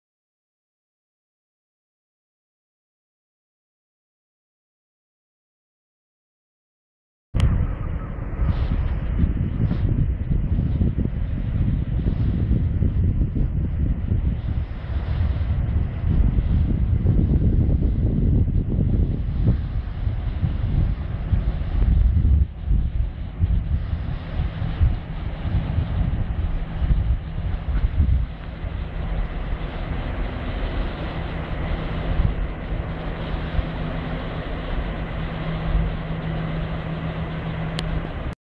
Schlepper "Axel" Vorbeifahrt 24/01/2013 in Rostock/Warnemünde (HD)
Der Lübecker Schlepper "Axel" in Rostock/Warnemünde.
41t-Pfahlzug